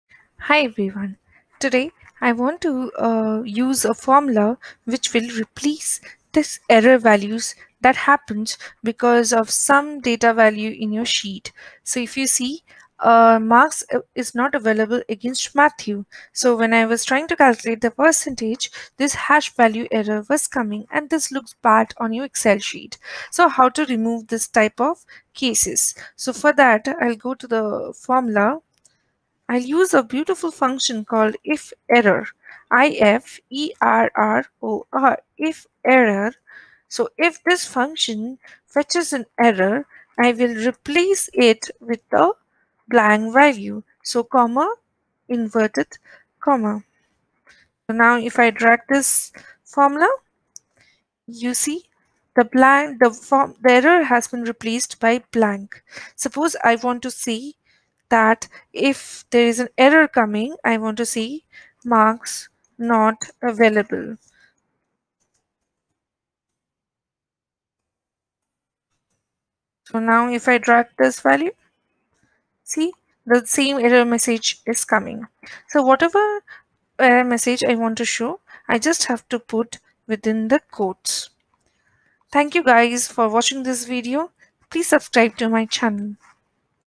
How to remove errors in excel formula & replace with blank value? Remove VALUE errors
Because of data issues, often VALUE error comes. This video talks about a simple formula "IFERROR" to replace errors in calculations with the desired text

How to fix the error?